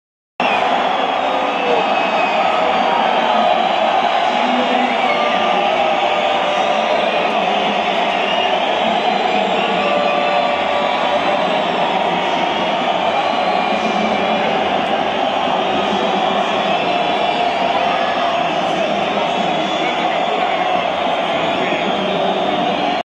Sampdoria-Juventus 2022/2023 - Fischi della Gradinata Sud alla lettura della formazione della Juve
I fischi della Gradinata Sud durante la lettura della formazione della juve prima dell'inizio della partita.